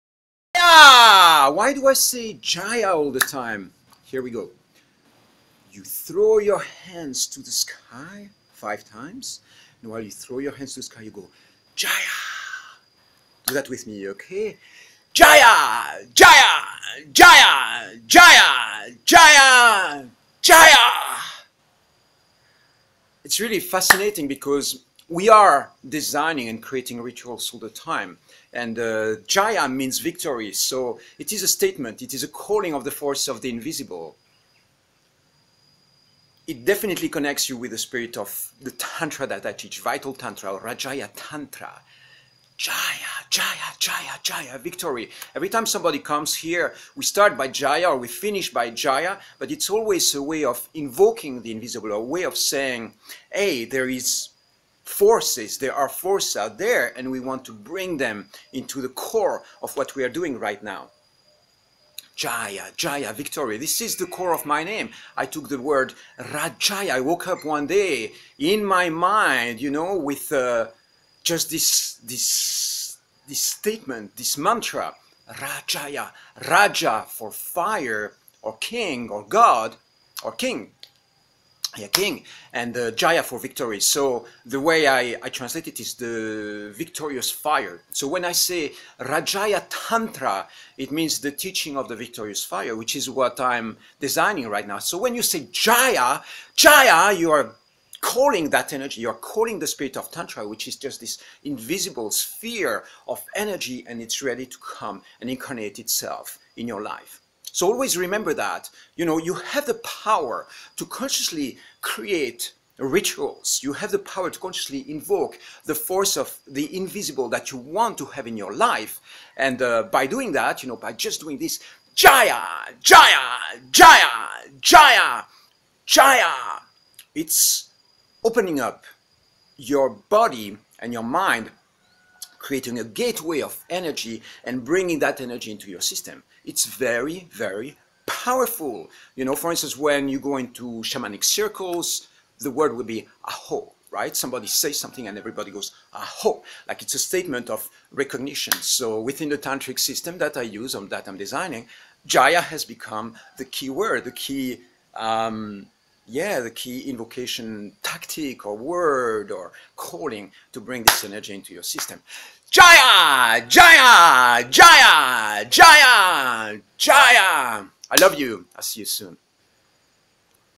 JAYA JAYA JAYA JAYA JAYAHAYAHAAAAA
Jaya!!! Victory!!! This is now a core mantra in my life! It's one of the core frequencies of my VITAL TANTRA system! Love this word so much that it is now embedded in my name "Shiva Rajaya" - I encourage you to use it frequently any time, greeting, waking up your fire and more! Love you!